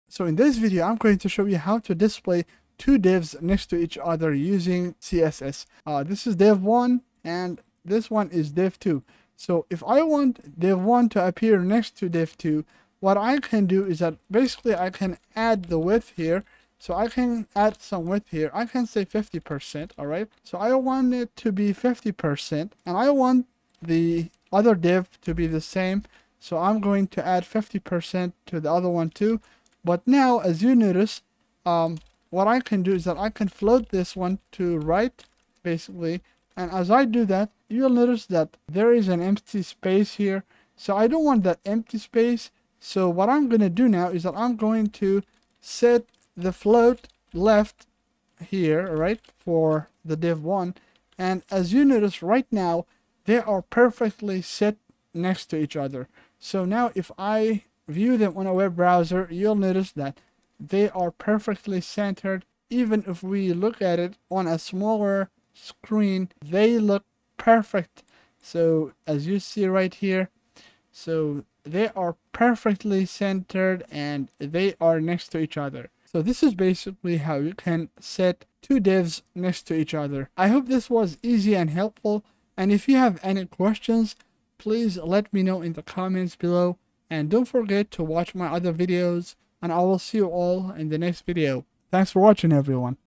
How to Place Two Divs Next to Each Other
In this video, I will show you how to put two divs next to each other using CSS